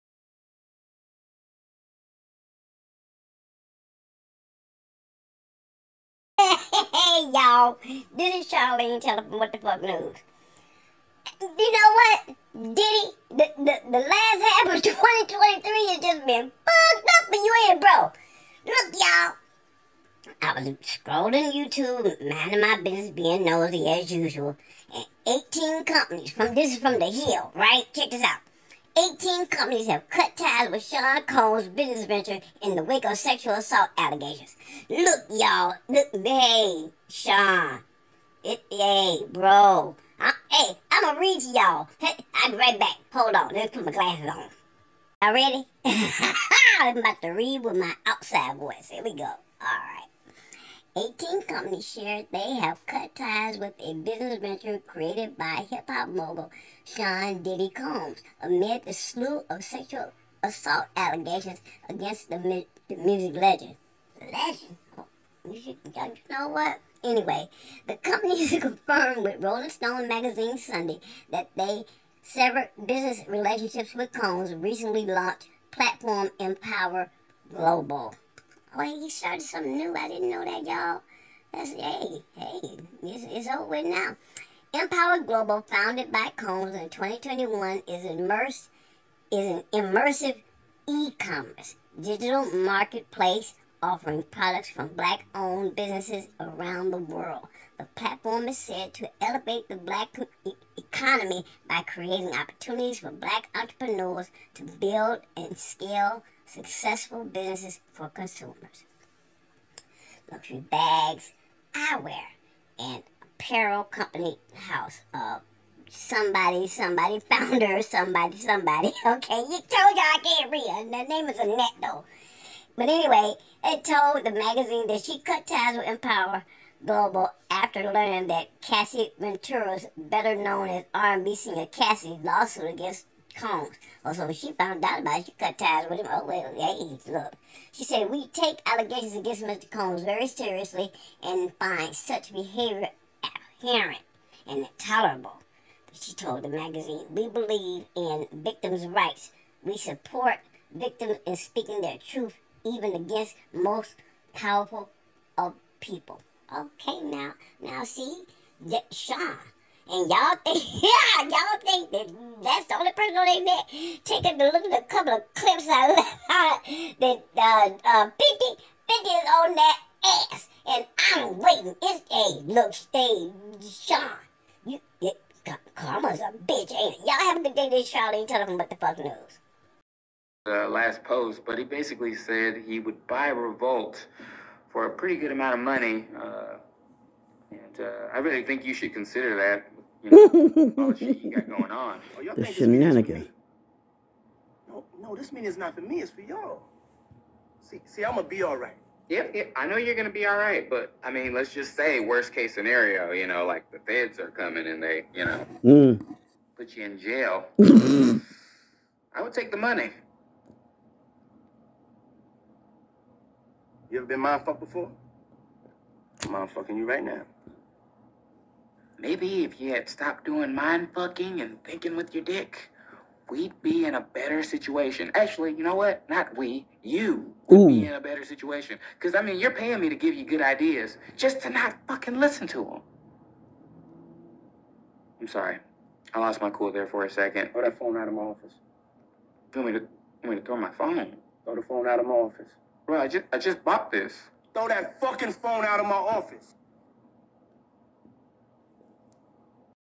18 companies have cut ties with Diddy
Music mogul Sean Diddy Combs is having a bad last half of 2023. Cassie's lawsuit against him has kicked open the floodgates, and everyone is leaving Diddy to deal with his karma, alone
18 companies have cut ties with Diddy amidst the slew of sexual assault and human trafficking allegations.
Credit Article: The Hill